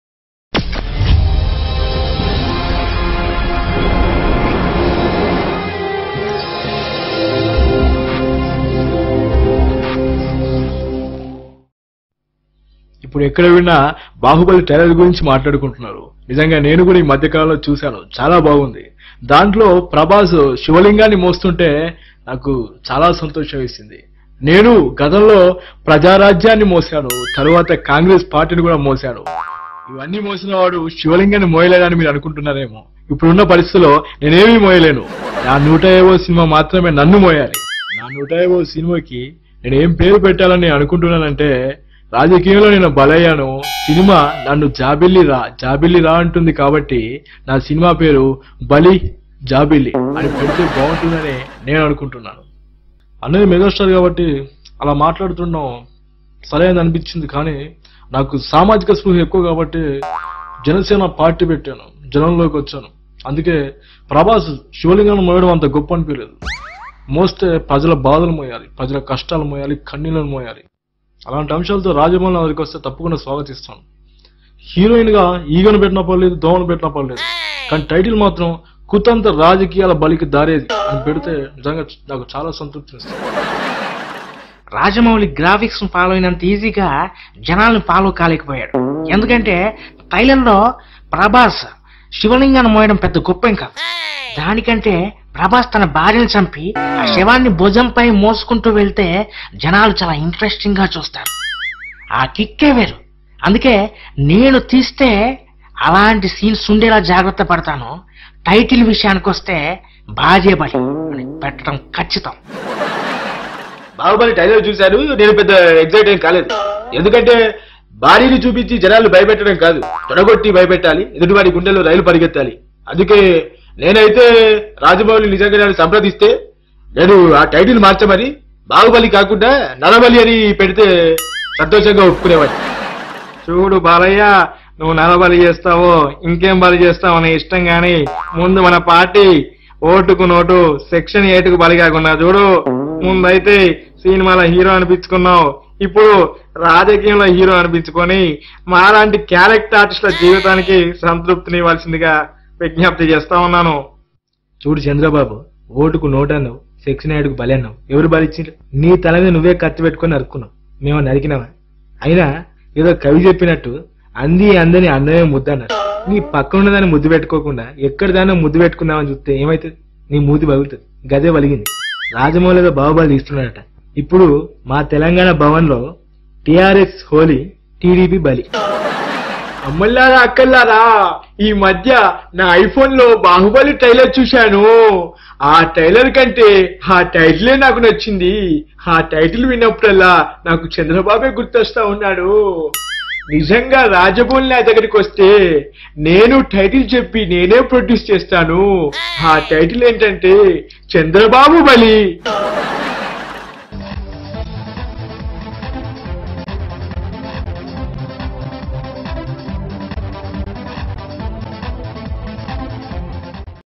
Celebrities Comments On Bahubali Trailer || Fan Made
Check out Baahubali trailer response by Celebrities like CHiranjeevi, Pawan Kalyan, Ram Gopal Varma, Bala Krishna, Nara Chandra Babu Naidu and KCR.